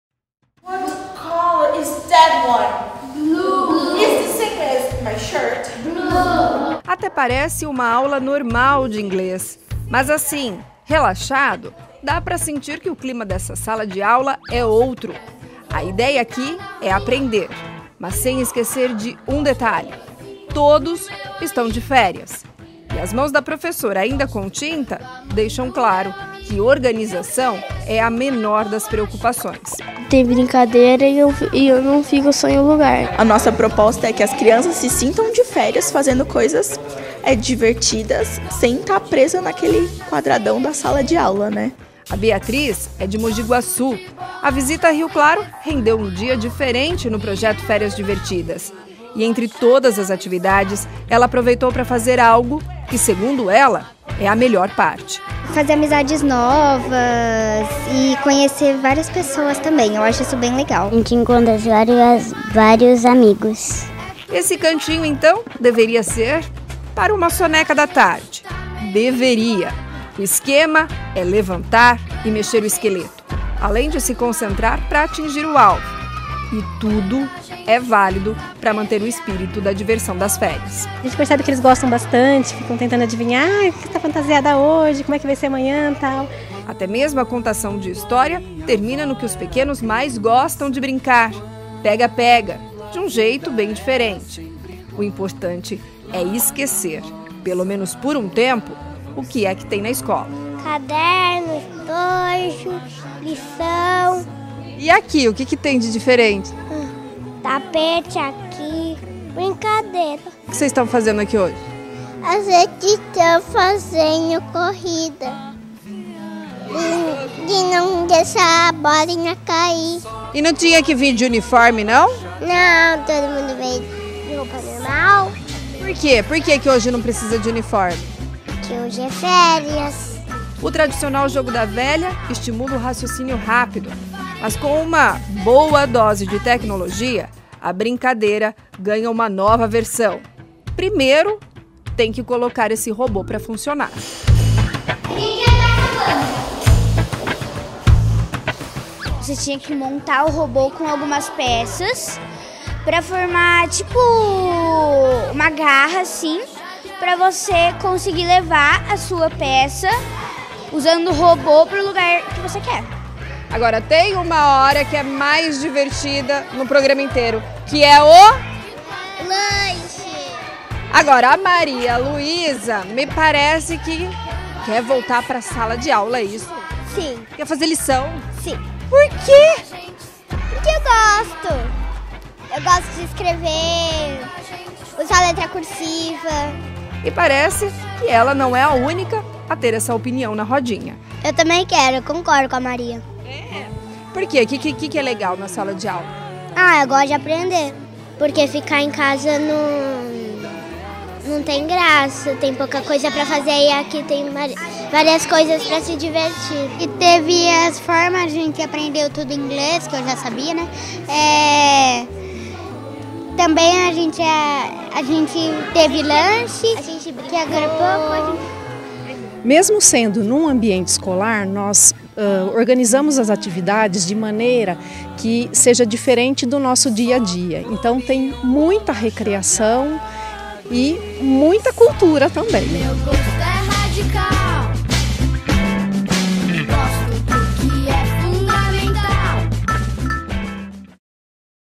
Férias na escola? Aqui sim, e divertidas!
Esta é a 4 ª Edição do Claretiano Férias Divertidas e é claro que a garotada se divertiu nas mais diversas atividades educativas e recreativas.